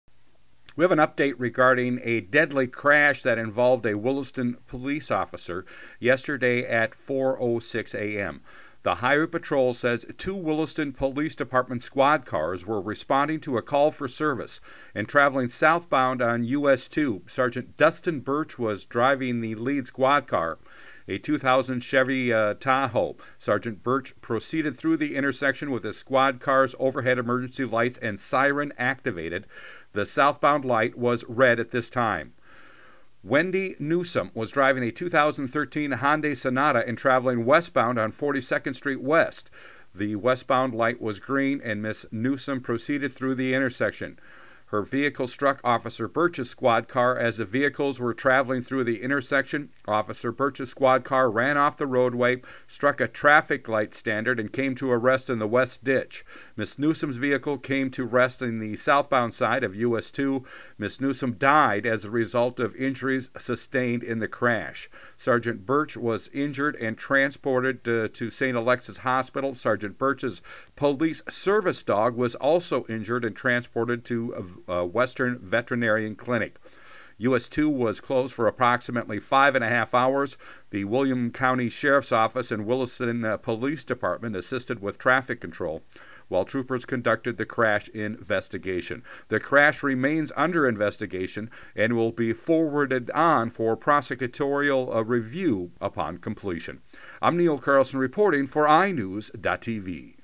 UPDATE: Details In Deadly Williston Crash Involving Police Officer
WILLISTON, ND (iNewZ.TV) Details regarding a deadly crash involving a Williston, North Dakota Police Officer Tuesday morning.